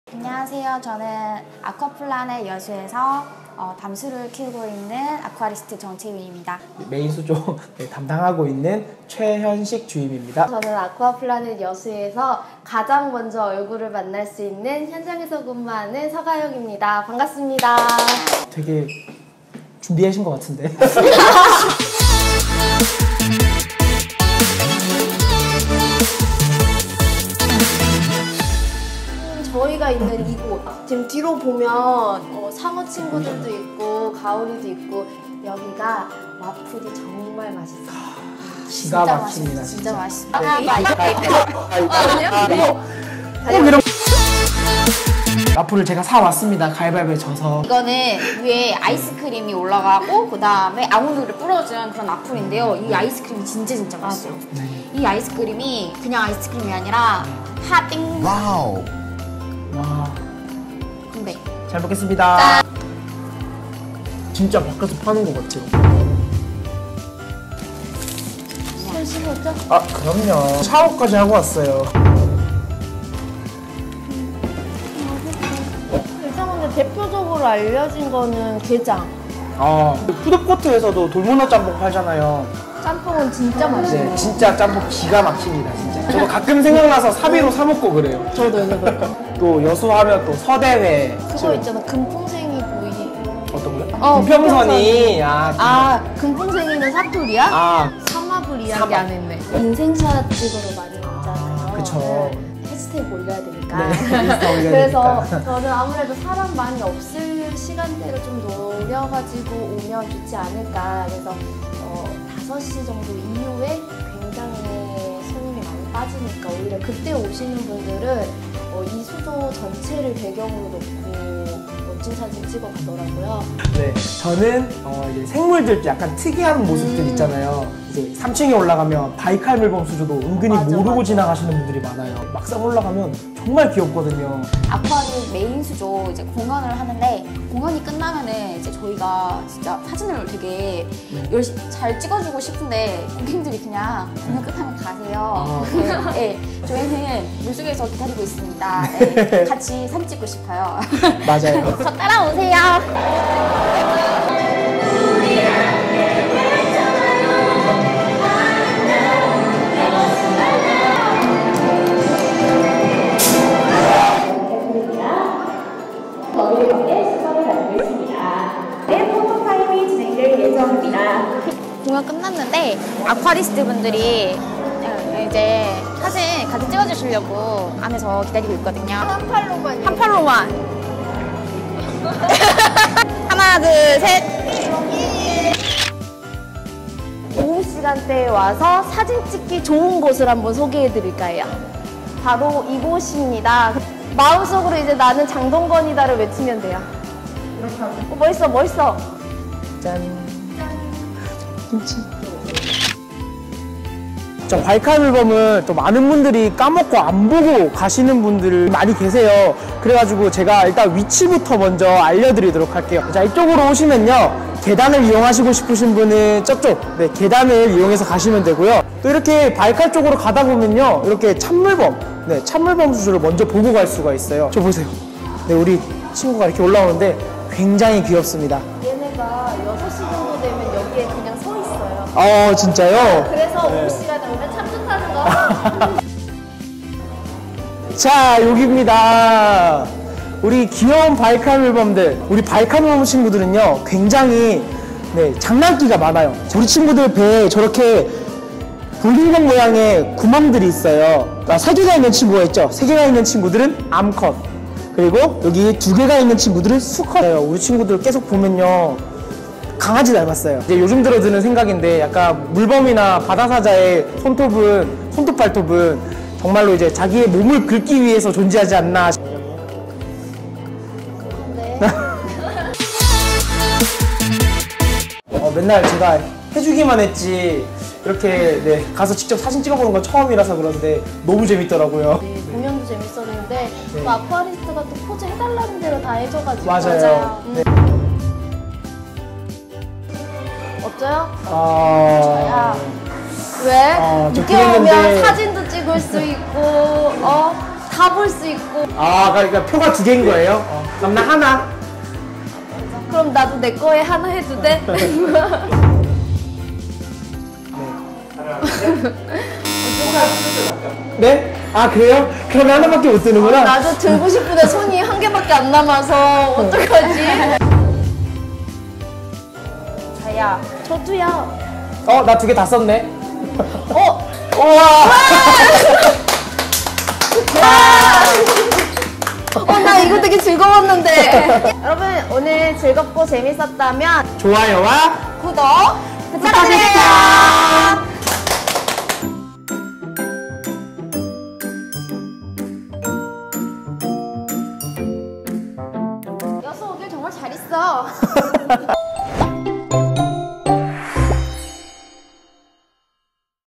진짜 직원이 알려주는 아쿠아플라넷여수 인생샷찍는법 대방출ㅋㅋ(ft.JMT와플)
직원들이 답답해서 알려주는 아쿠아플라넷 여수 꿀팁대방출!

아쿠아플라넷 여수 놀러가는 사람들은 필수시청하세요~~
메인수조 보면서 아이스크림 올려먹는 와플 강추!
그리고 다이버와 함께찍는 인생샷 꿀팁추천~

누구 팁이 더 꿀팁인지 겨루는게 꿀잼포인트ㅋㅋ